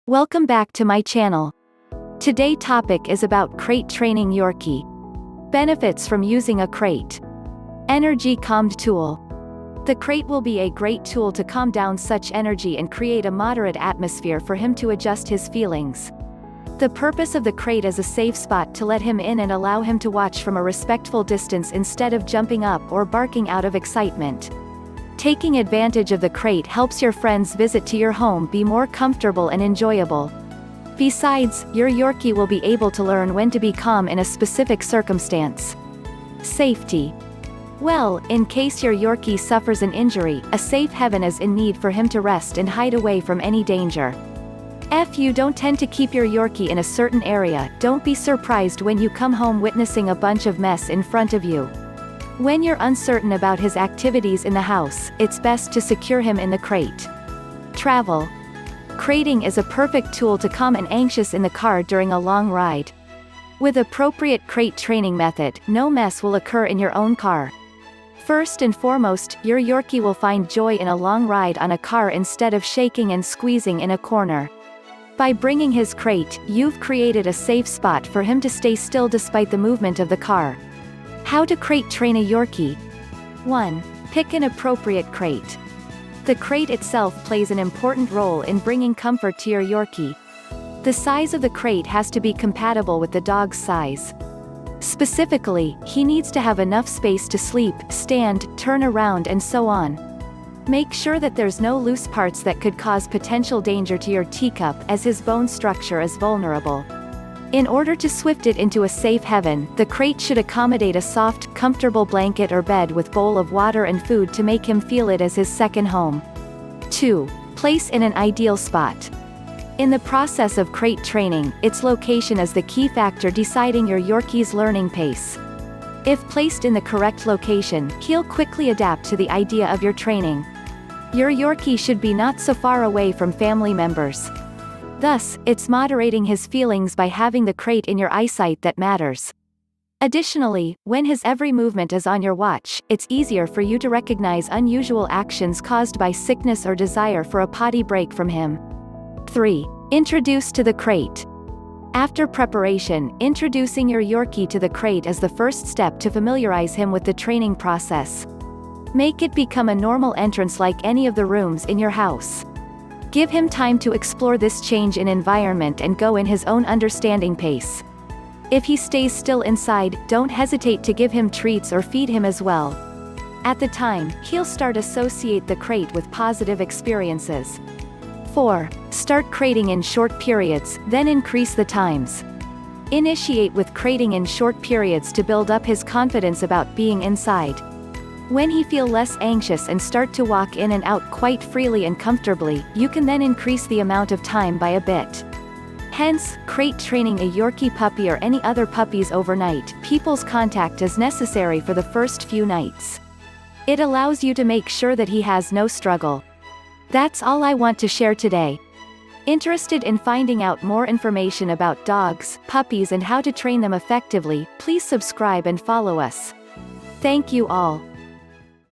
Crate Training Yorkie – A Challenge Or A Piece Of Cake?
Click to read the whole blog:
When you're away from home or in bedtime, you don't want your Yorkie to walk around the house and create any mess? It's time for crate training a Yorkie. Don't hesitate no more. Let's watch!